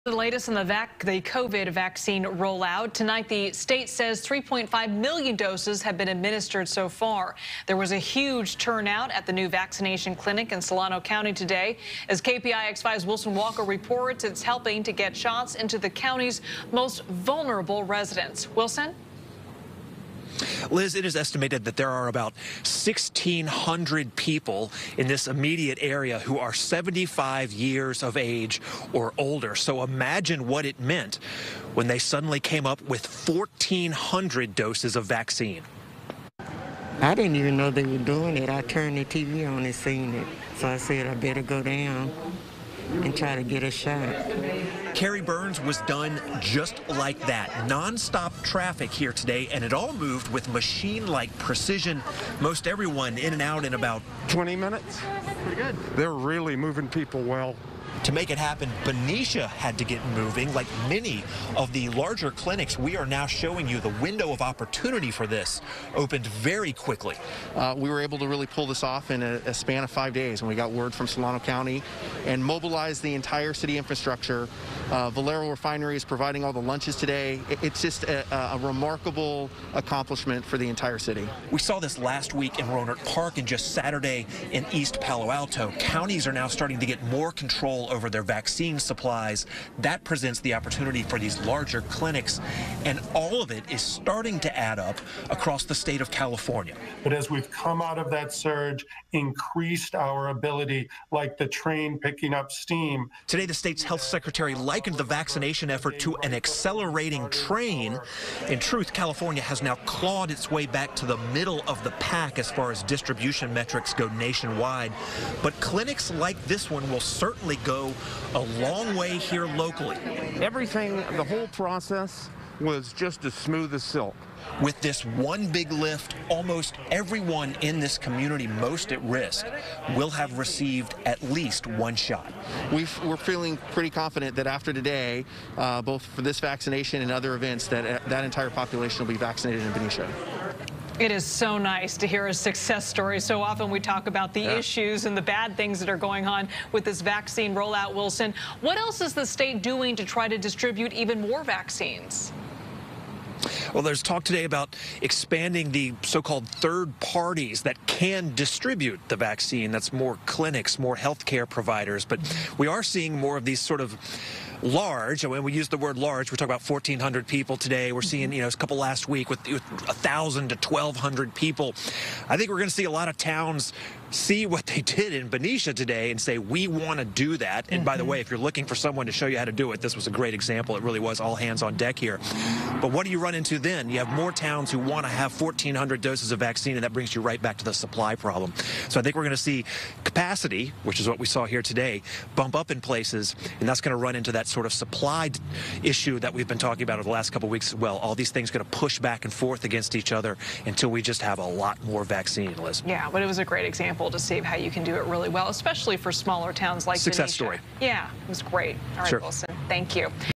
COVID Vaccines: Benicia Moves Fast On Distributing 1,400 Shots
In Benicia and the immediate surrounding area there are thought to be about 1,600 people 75 years of age or older. It was monumental news when this community suddenly received 1,400 doses of vaccine. Wilson Walker reports. (2/2/21)